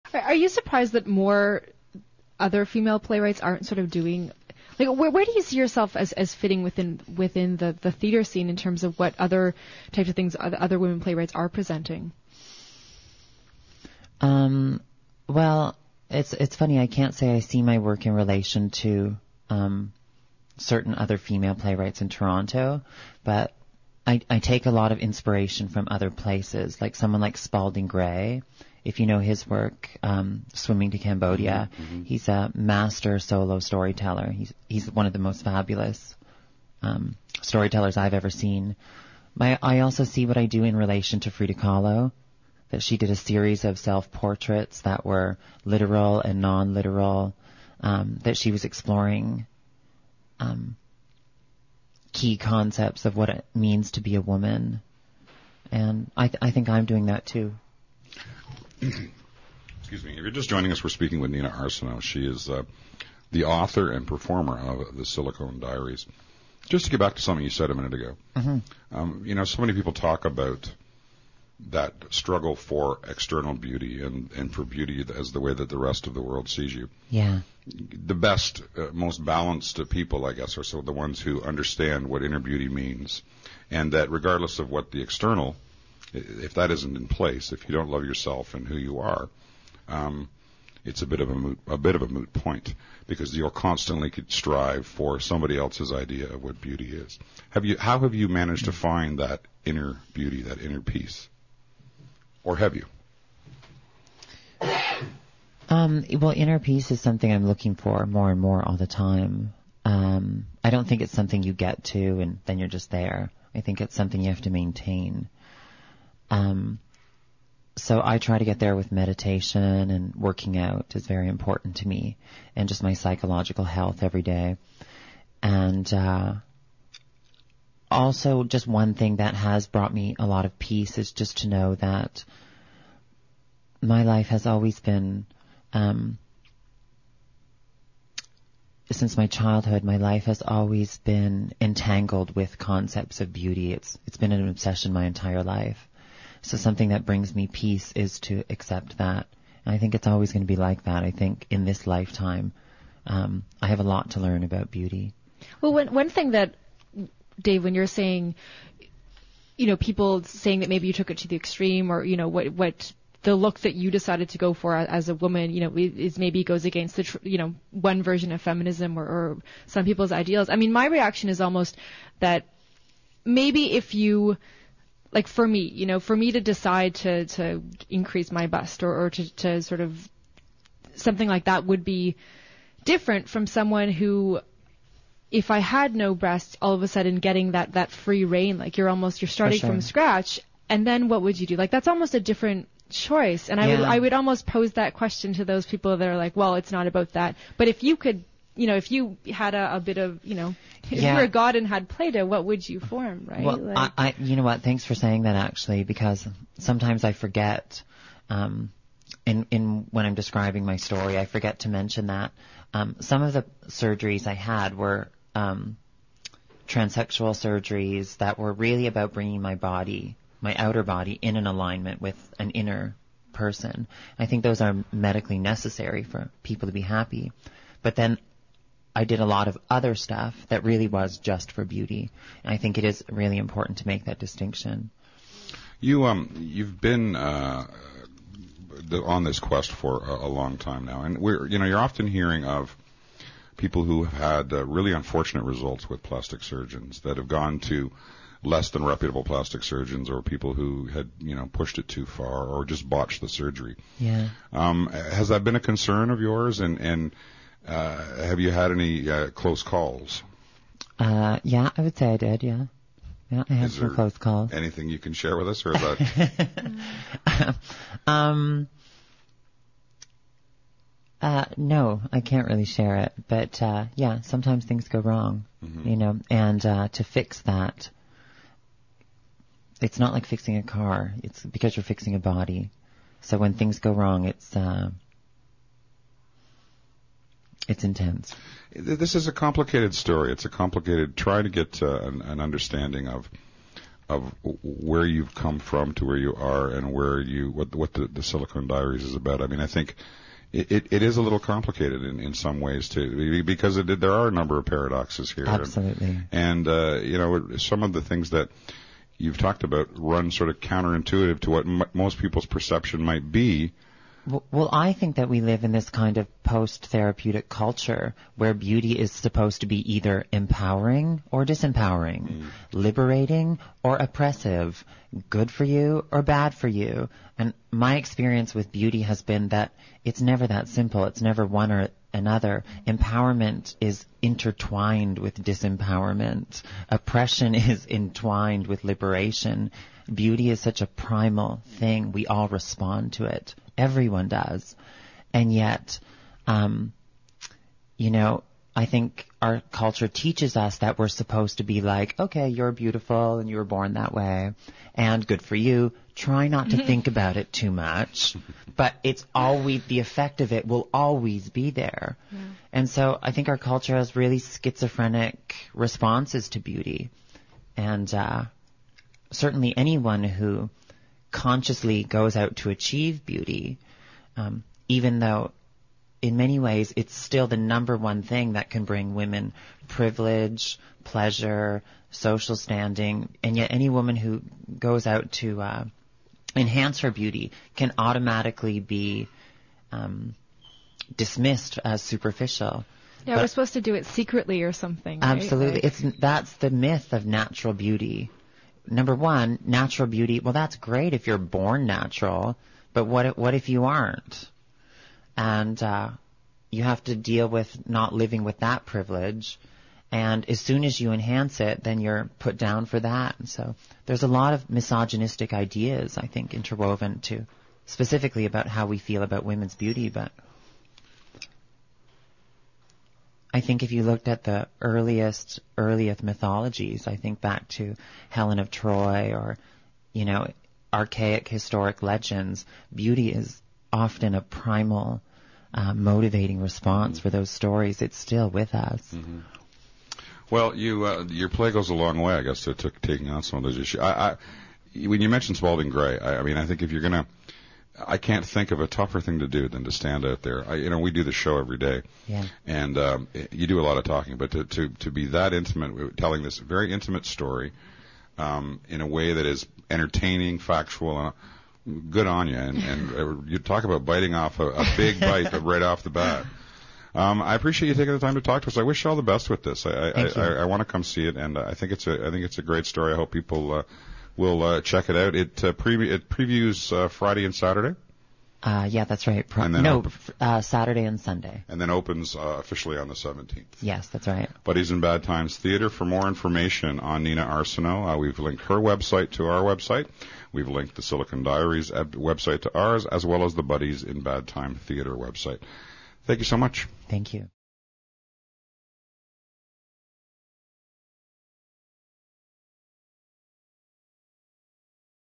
Trans-artist Nina Arsenault on U of Toronto radio (Part 2 of 2)
CIUT radio interview to talk about The Silicone Diaries, my solo performance piece presented at Buddies in Bad Times Theatre, Nov 2009